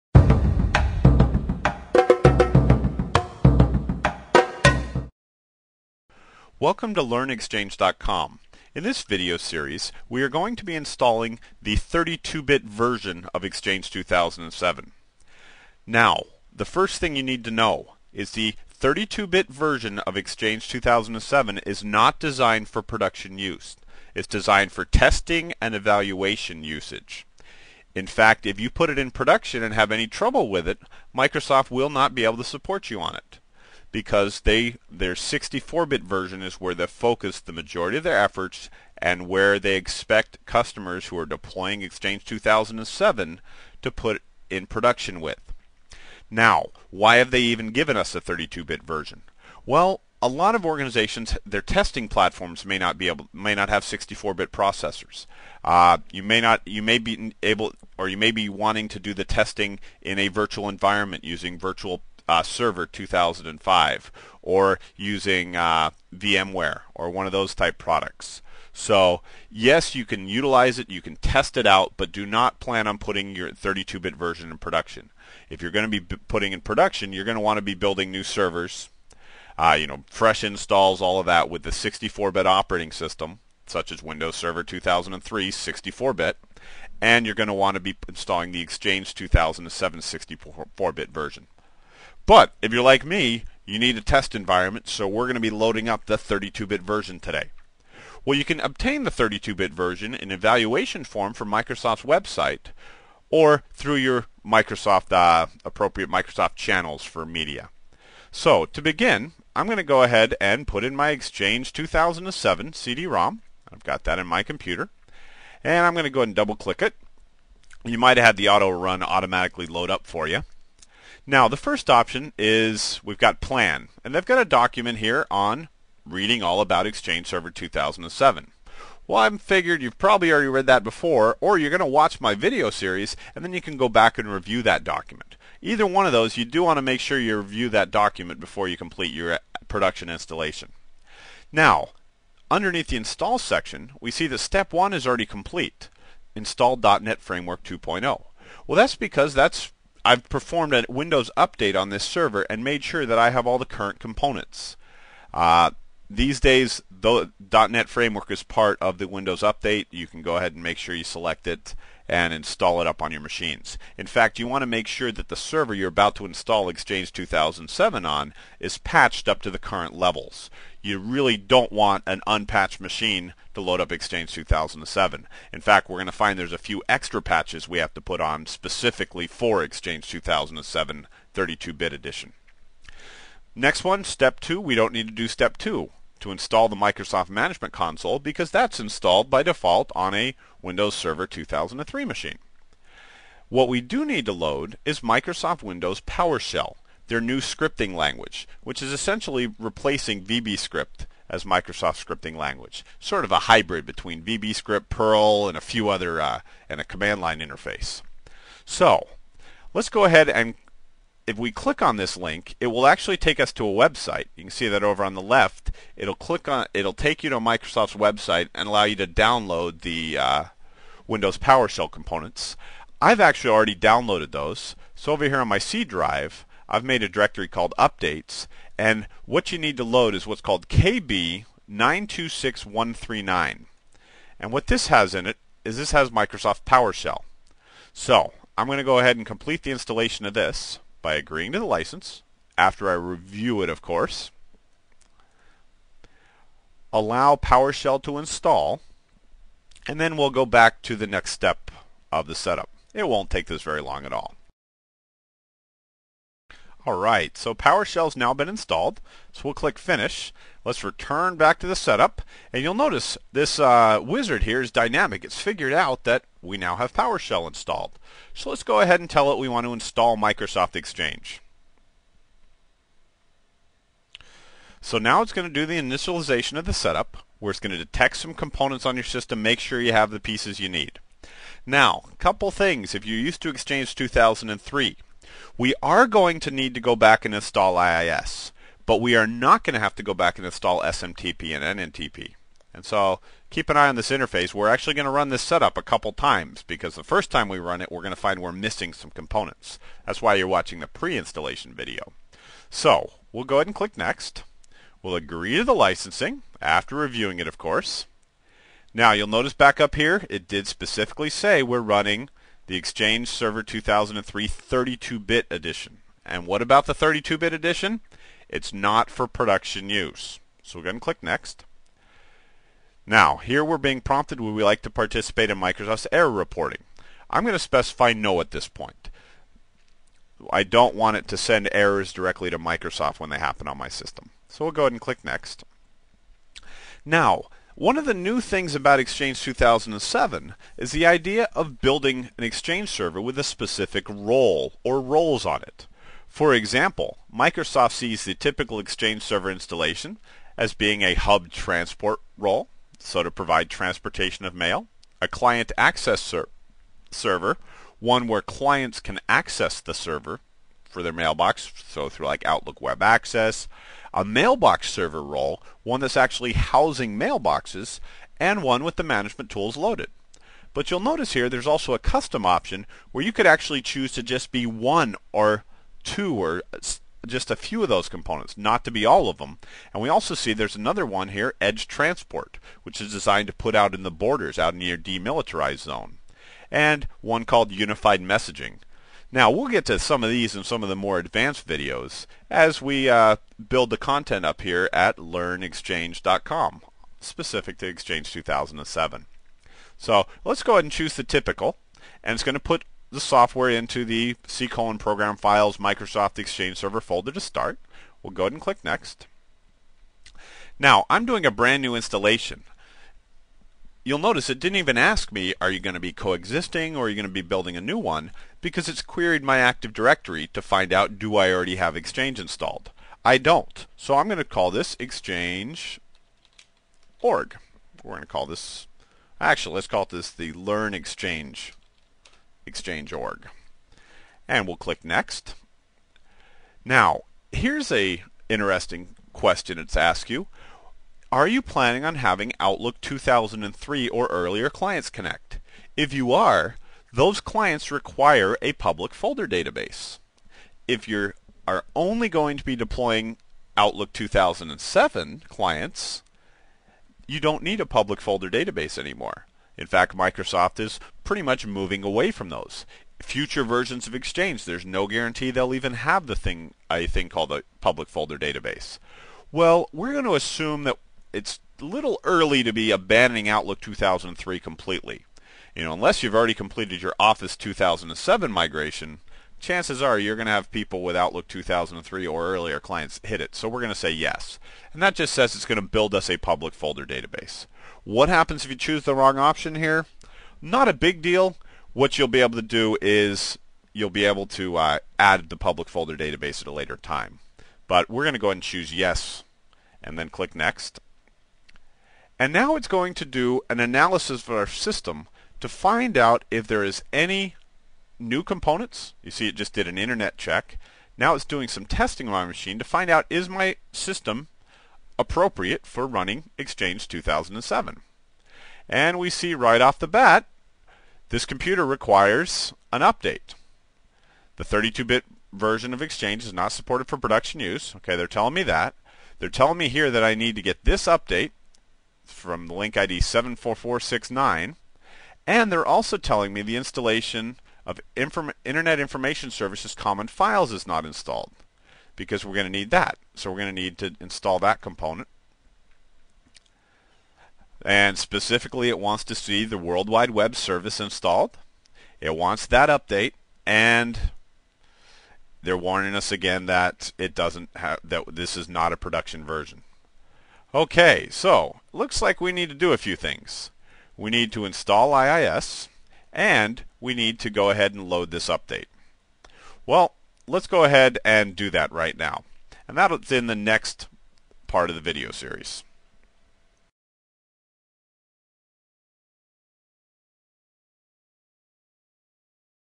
Exchange 2007: An Overview of the Setup Process
Visit LearnItFirst, our new channel with over 100 videos!

This video, the first in our series on installing Exchange 2007 (32-Bit) talks about what the purpose of the 32-bit version as well as walks you through the setup process, starting with the Readiness Checks. The Exchange 2007 setup is a lengthy process that can take an hour or more so it's best to have a solid introduction like this one.

Author: Grant Moyle